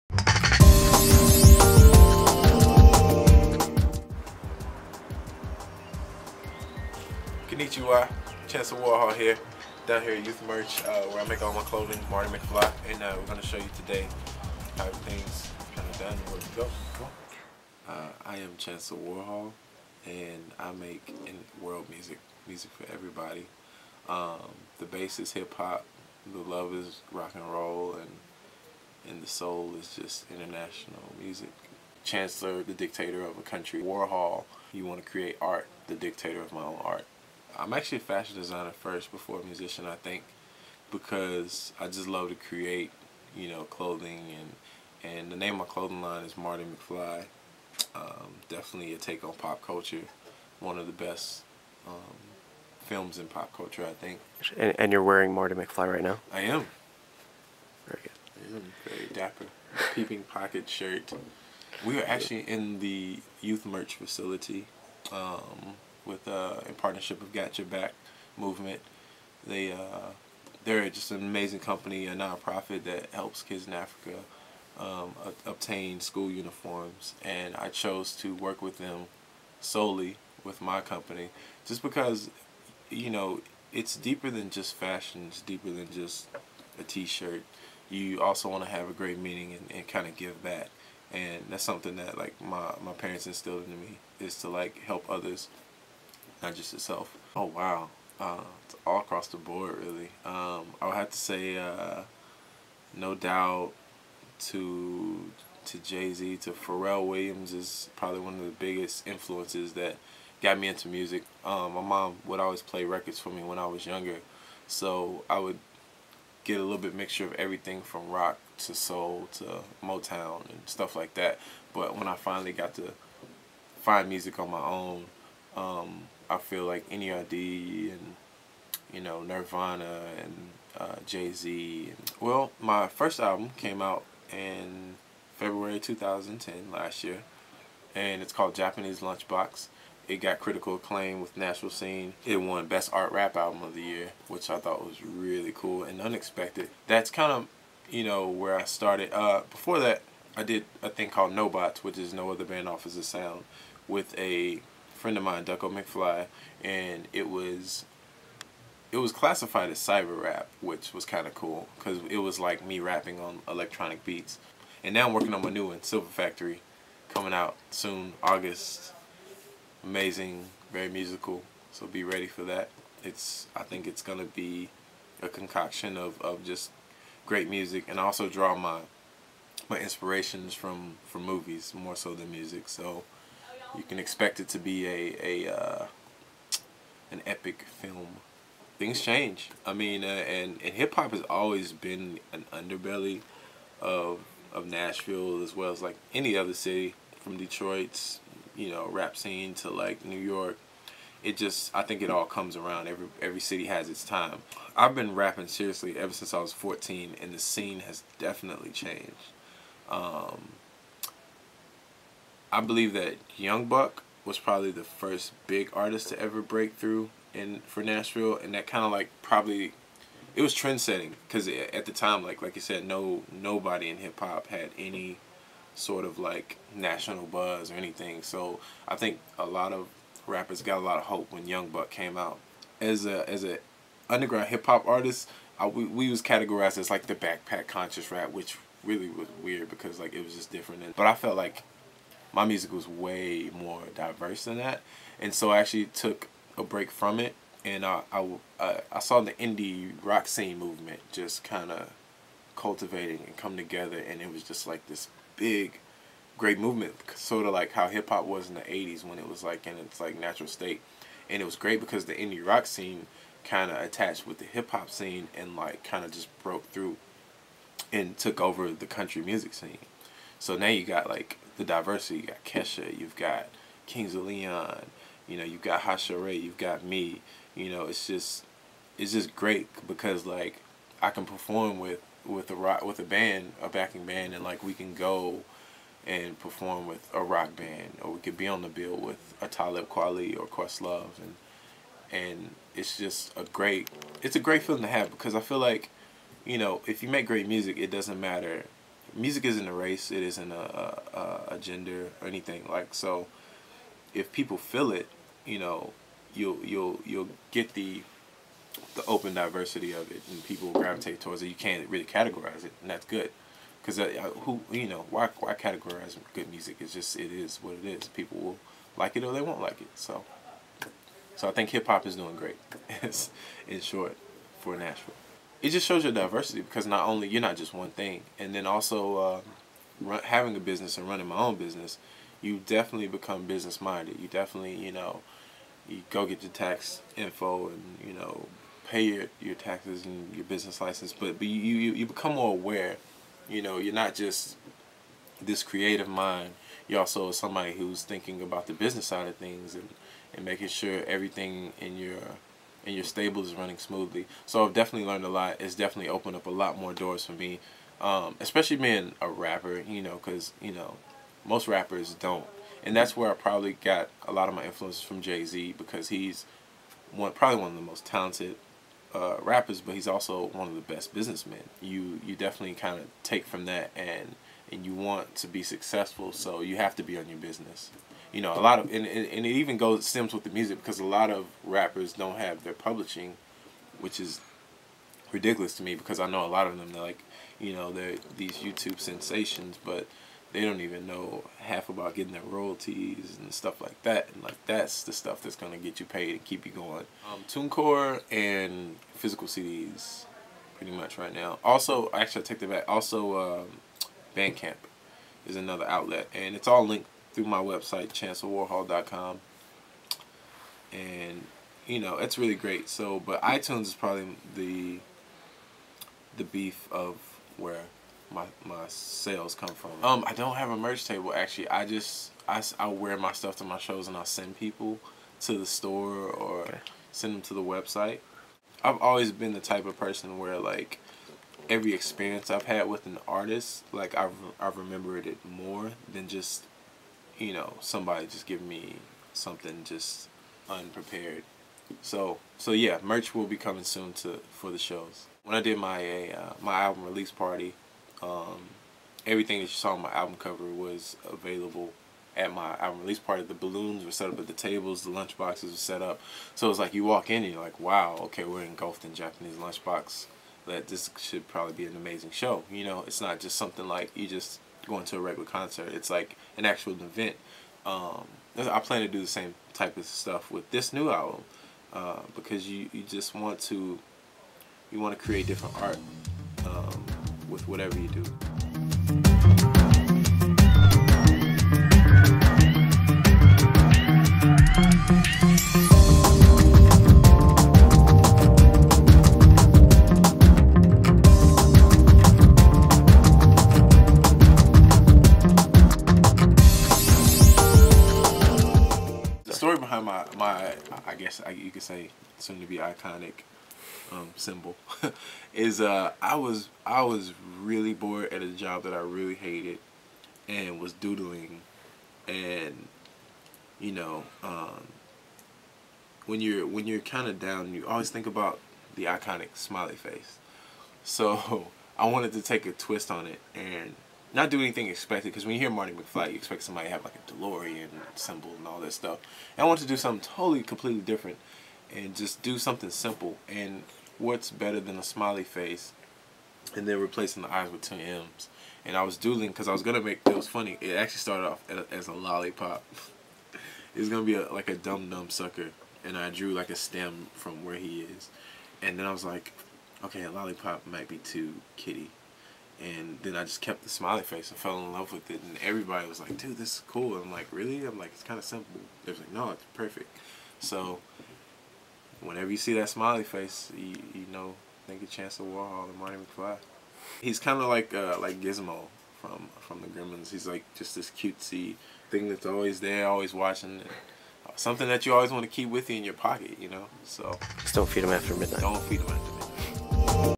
Creators' Freedom Project Introduces Chancellor Warhol
Creators' Freedom Project intro interview with Chancellor Warhol. Nashville-native hip hop artist and fashion designer is participating in the Creators' Freedom Project case studies to help artists make a living from their creativity by strategically using the tools of today.